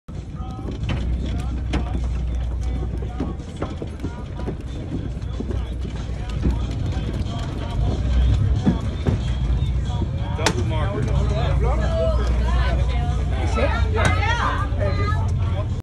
Pacific Swordfish caught right between us and Catalina Island ⚔️
Jon’s Fish Market welcomes you to enjoy their seafood, fish market, and patio!

Our Address:
Jon's Fish Market